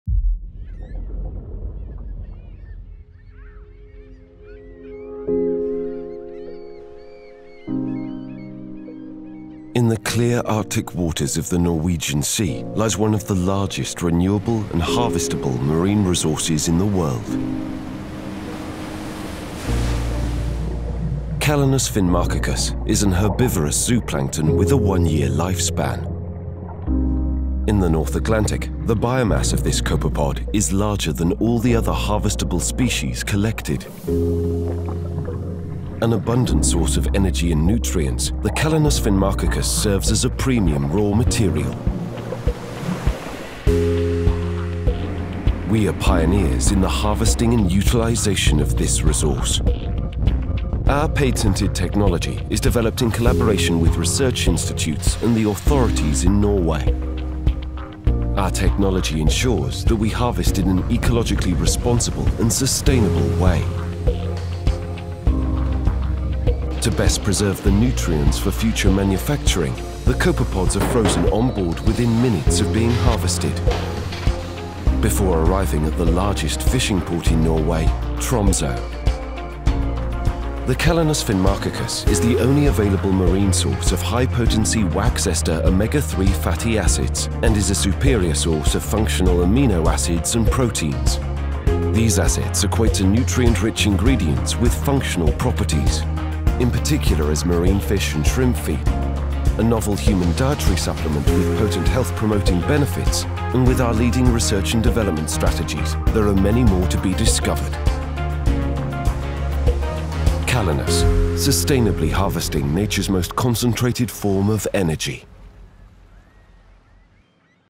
Zooca® - The Calanus Company - sustainably utilizing a new vast resource
Sustainable harvesting of Calanus finmarchicus, the premium raw material used for development of high-end health and nutrition products for humans and animals.